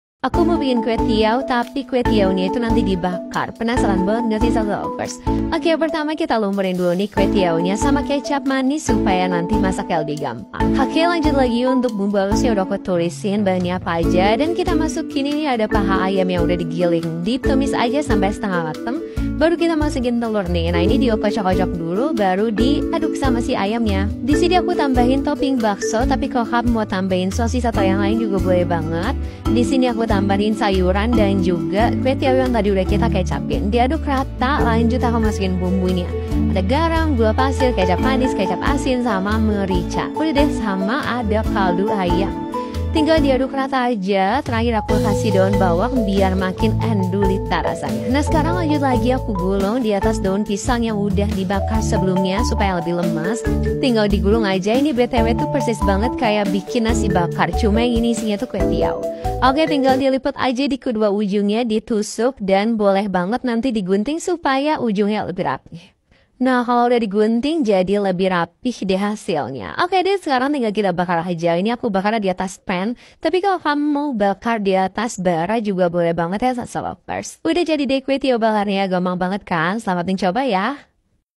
IDE JUALAN SUPER CUAN! KWETIAU BAKAR SUPER ENDUL!
Jualan nasi bakar udah biasa, nah ini dia kwetiau bakar bisa jadi ide jualan yang super cuan!

Produser Eksekutif: Intan Yusan Septiani
Penulis Resep: Indri, Resi, Qorin, Ruth
Penulis Naskah: Rika
Peraga: Resi, Indri, Qorin, Ruth
Fotografer: Andrian, Osyad, Bram, Yudi
Videografer: Andrian, Osyad, Bram, Yudi
Video Editor: Andrian, Osyad, Bram, Yudi